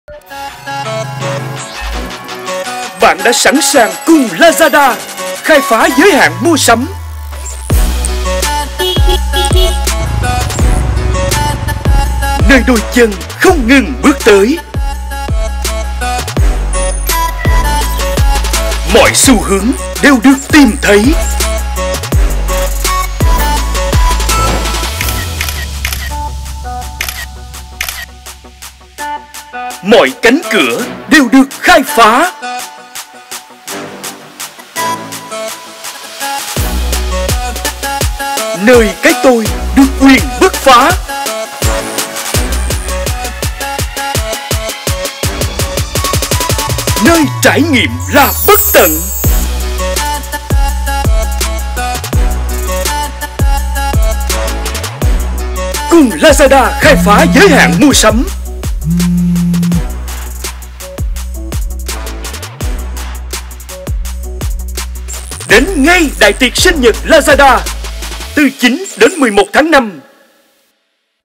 Lazada.vn | 6 Năm Khai Phá Giới Hạn Mua Sắm
Để kỉ niệm cột mốc 6 năm đáng nhớ này, Lazada tưng bừng tổ chức Đại tiệc Sinh Nhật vào 3 ngày 09-11/05 với ưu đãi siêu khủng:
🔴 Giảm giá đến 50%
🔴 Deal chớp nhoáng giá chỉ 6.000Đ
🔴 Voucher đến 6 Triệu Đồng
🔴 Gửi điều ước và nhận quà đến 50 Triệu Đồng